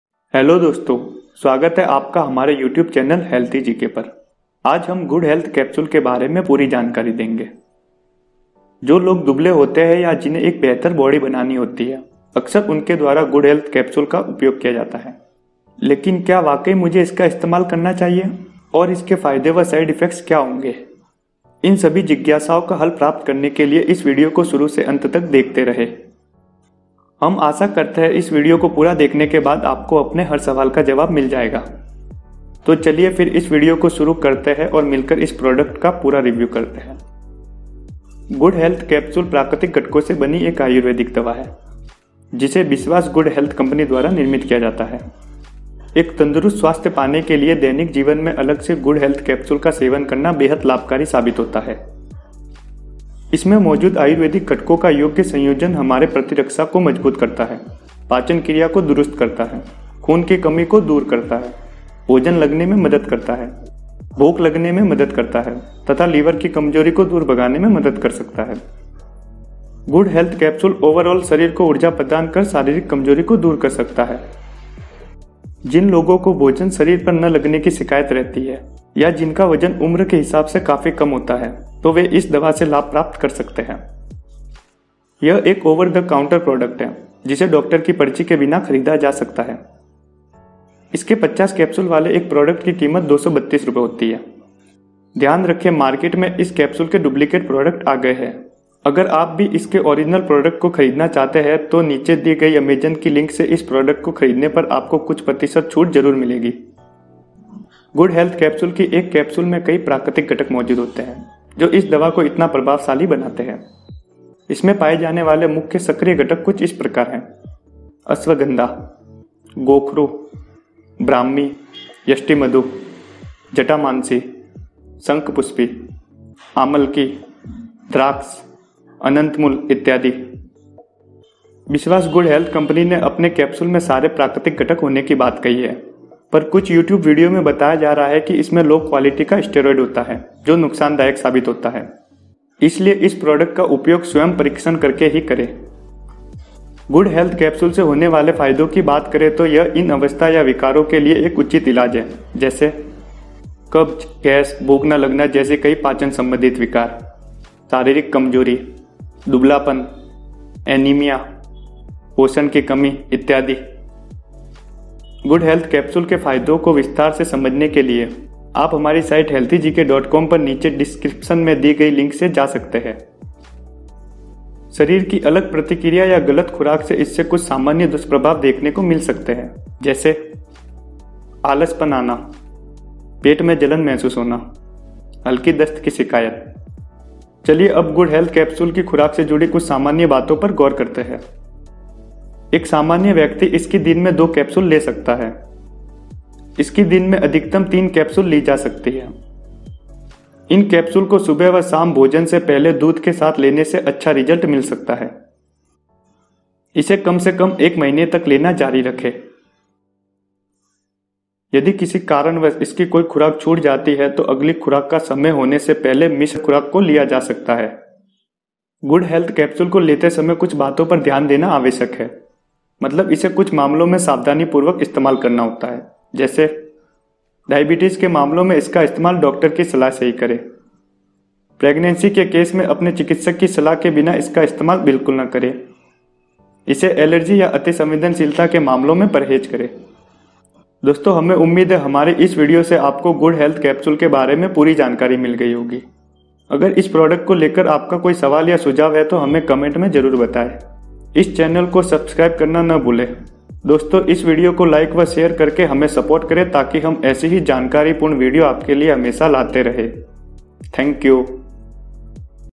गुड हैल्थ कैप्सूल के फायदे, नुक़सान व खुराक | Good Health Capsule Review, Uses, Side Effect in Hindi
This video is a Good Health Capsule Review in Hindi. Good Health Capsule is suggested to increase body weight and obtain a better physique. Watch this video till the end, to know the benefits, side effects & dosage of Good Health Capsule.